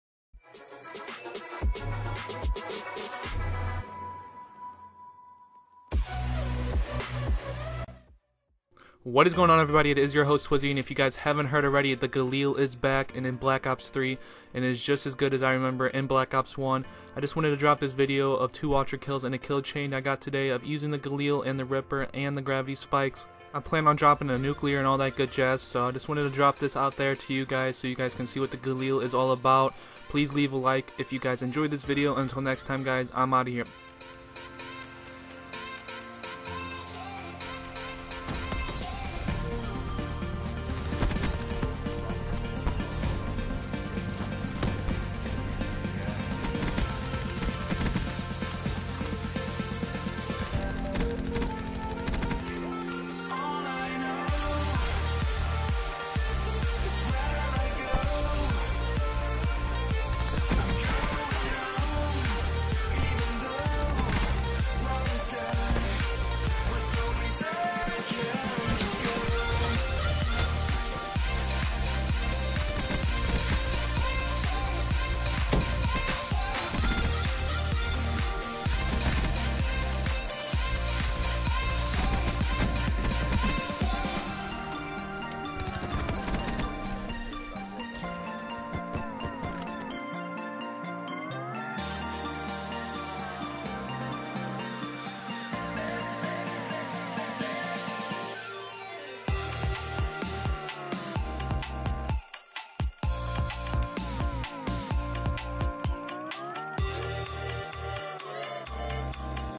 The Galil is BACK! 2 Ultra Kills & Kill Chain
New galil gameplay (Ultra kill & Kill Chain)